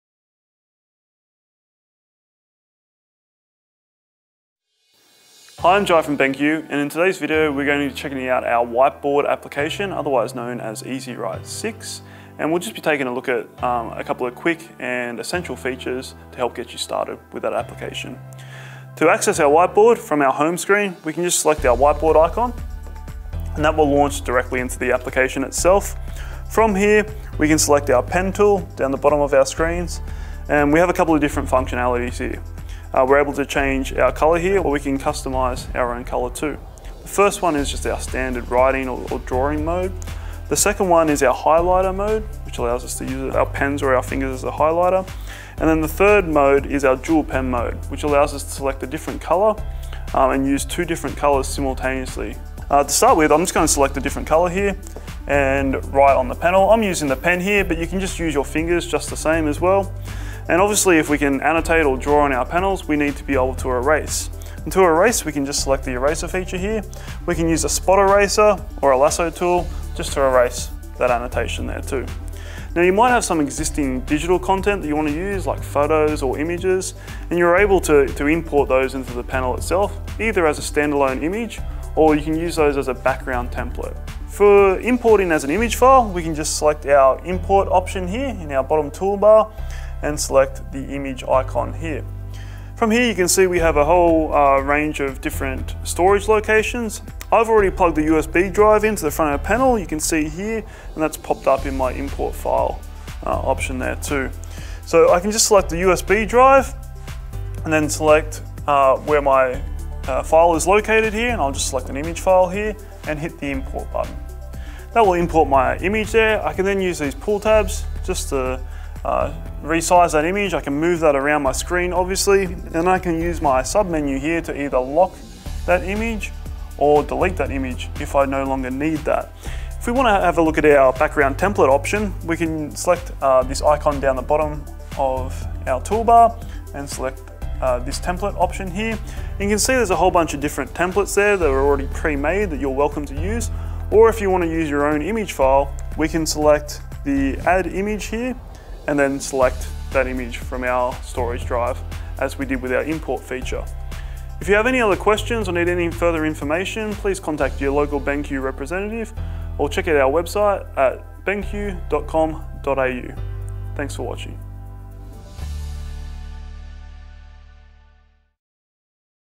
Interactive Display Onboarding Video - EZWrite 6 Whiteboard Essentials | BenQ Education
Let us help you get started with EZWrite 6 Cloud Whiteboard Essentials!

The all-new BenQ EZWrite 6 Whiteboarding Software allows you to unleash your creativity with a wide variety of teaching tools and features, ready to take your classroom experience to the next level!

Chapters:
0:00 Introduction
0:28 Annotation/Pens
1:05 Eraser
1:24 Importing Files
1:37 USB drive access
2:12 Background template option